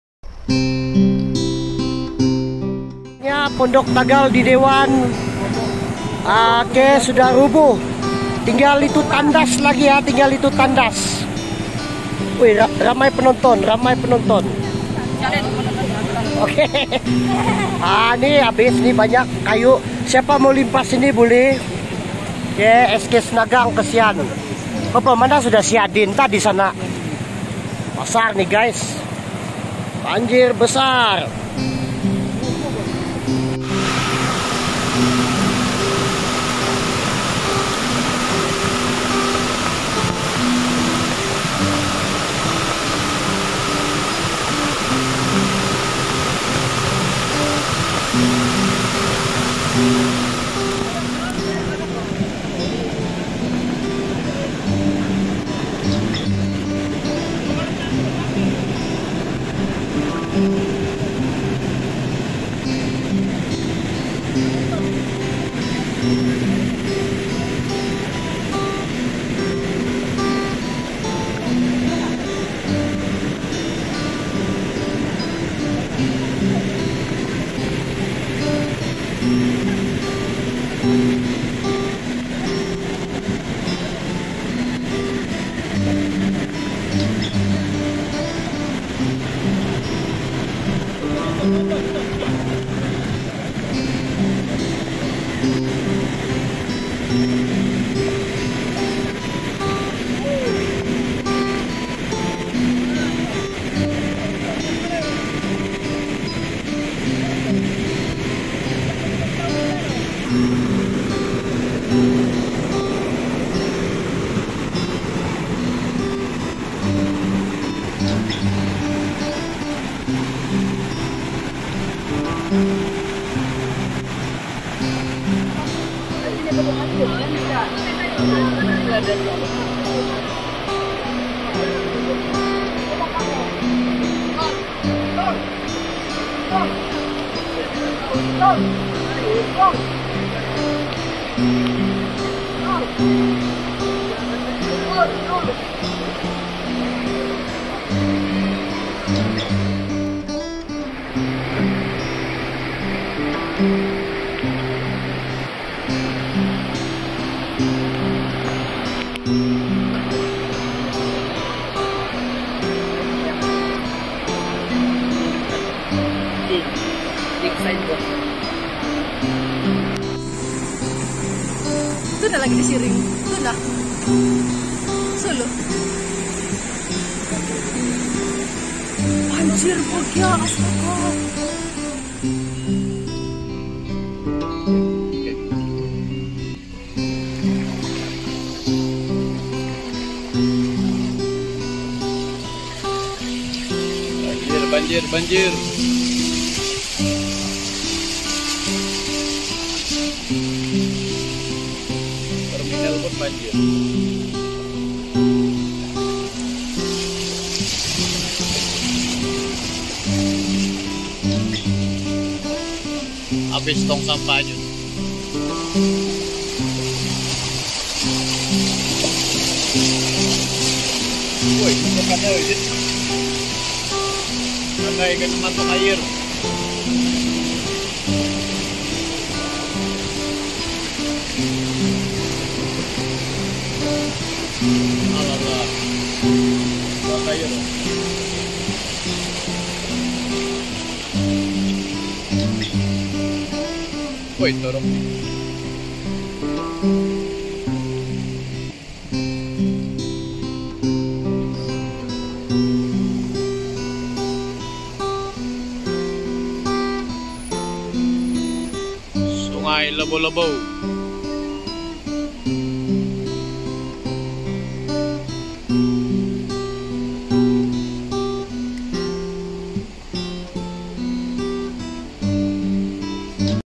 BANJIRRR!!!Awas waspada!!Antara Senario banjir di pedalaman Sabah pada 31/10/2022#banjir#tambunan
situasi banjir disabah pd 31/10/2022